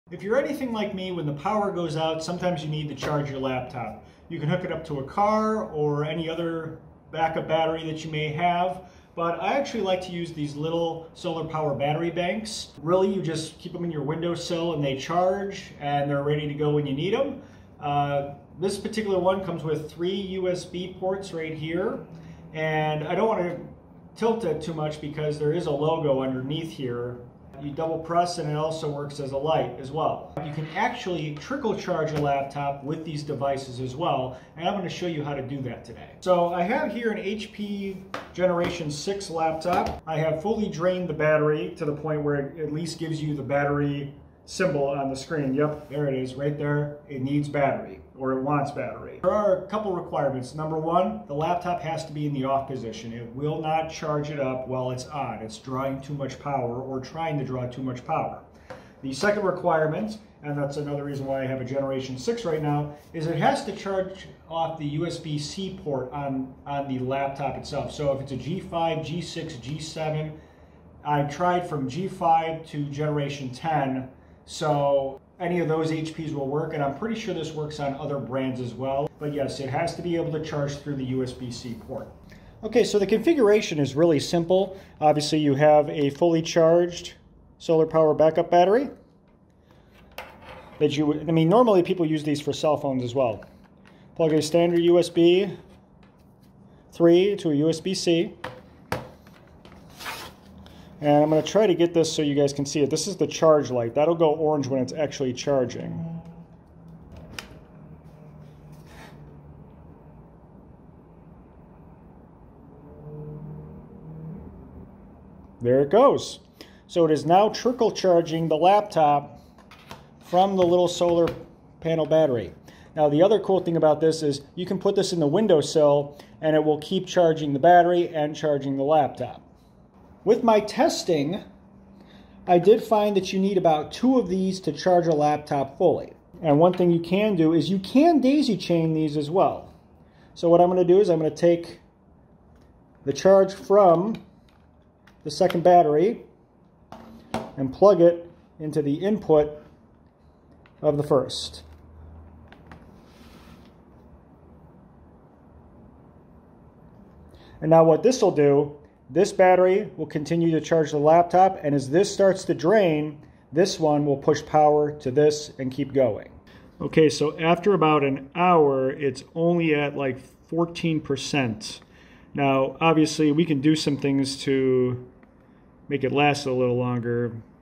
💻 Trickle Charging Your Laptop Anywhere!
Today I'm going to show you how to trickle charge a laptop with a small solar powered battery pack. This is perfect for traveling, camping or even power emergencies.

Requirements:
1. You must be able to charge the laptop via a USB-C port
2. Laptop must be off
3. Have a USB 3 to USB-C cable

These small solar battery packs are also great for phone and extra power for things like the Meta Quest 3. I keep one in my VR bag at all times.

Pick one up for yourself